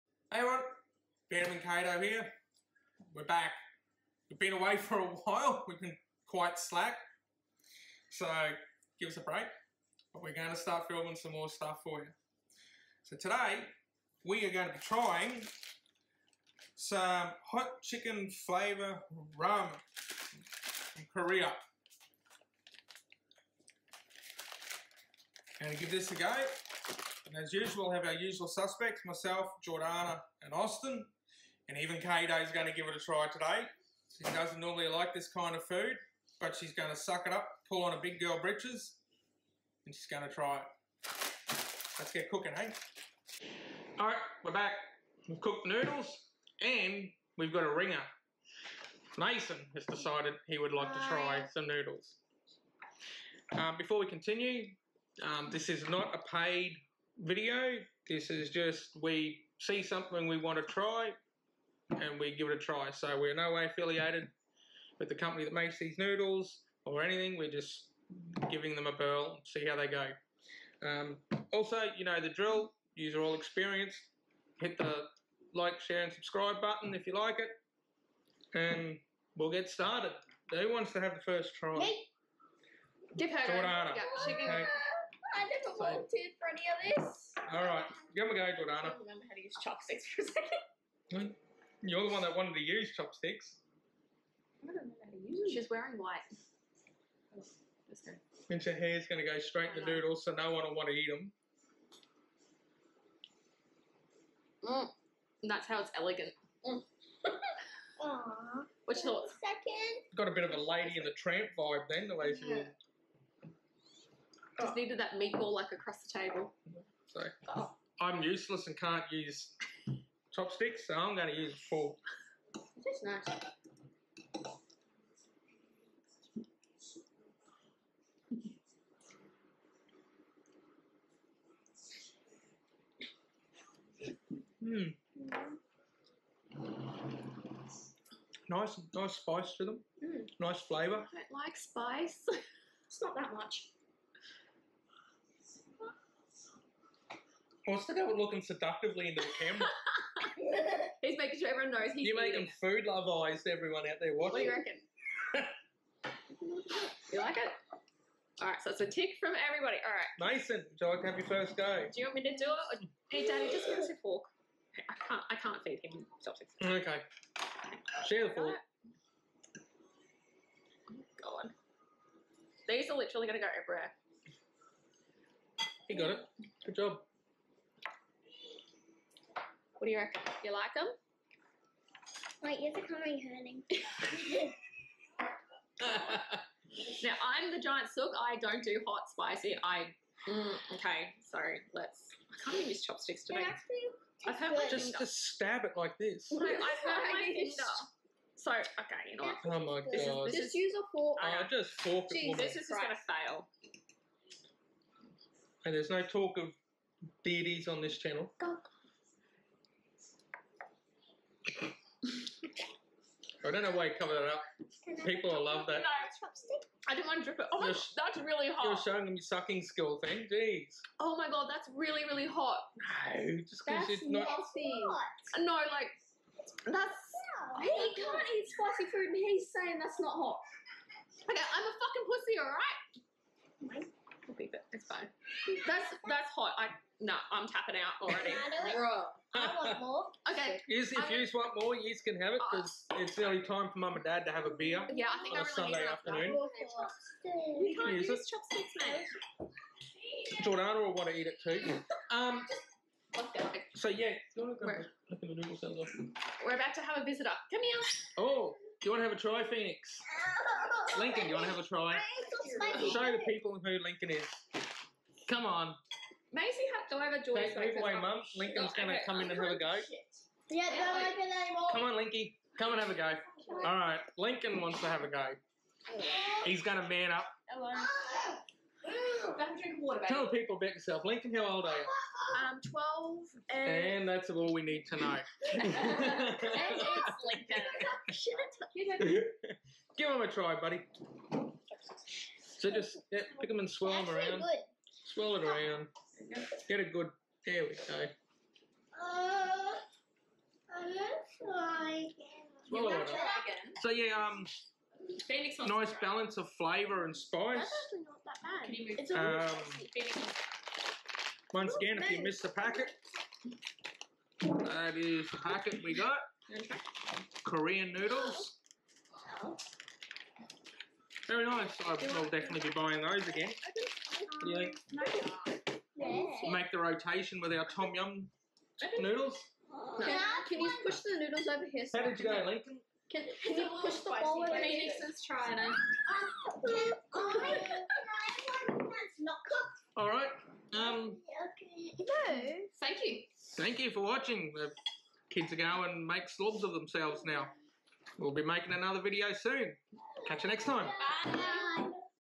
Family taste test - Buldak Carbonara Hot Noodles
Bantam & Kato are joined today by the rest of the kids to try some new Hot Korean noodles for the first time. Only a couple like hot and spicy stuff and Kato is a giant sook when it comes to hot/spicy foods as you may very well see.

Do you have a favourite hot foods that we should try?